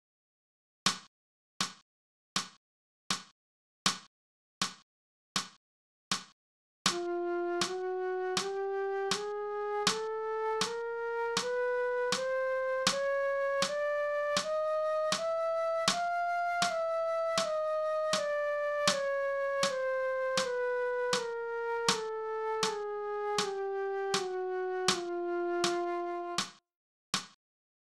Flute Chromatic Scale (F-F) Practice in Quarter Notes (Primary Ocatve)
Practice Goals:
1. Read/Say the note names while watching the video (3 times)
2. Read/Say the note names while fingering the correct notes while watching the video (correctly 3 times)
3. "Air-Band" the notes (tonguing up & slurring down) while watching the video (correctly 3 times)
4. Play the scale (tonguing up & slurring down) while watching the video (correctly 3 times)
5. Do step number 4 from memory with the video, without watching the screen (correctly 3 times)